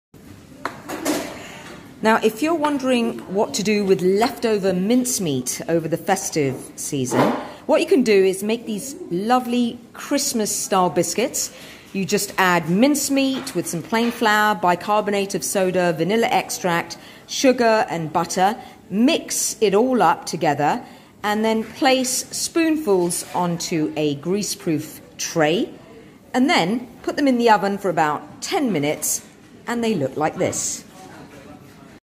Mincemeat Cookies using leftover mincemeat
Mincemeat  Cookies

Makes 12

125g margarine or butter
100g brown sugar
1 egg
1 tsp vanilla extract
5 tbsp mincemeat
220g plain flour
1/2 tsp bicarbonate of soda

Heat the oven to 180C/350F/gas 4. Beat the butter with the sugar. Beat in the egg, vanilla extract and the mincemeat. Stir in the flour and bicarbonate of soda, then chill for 20 minutes.
Scoop tablespoons of the mixture onto two lined baking sheets, leaving a bit of space between each so they can spread nicely. Bake for 10-12 minutes, or until the cookies are just firm around the edges but still a little soft in the middle – they will firm up as they cool. Transfer to a wire rack and leave to cool.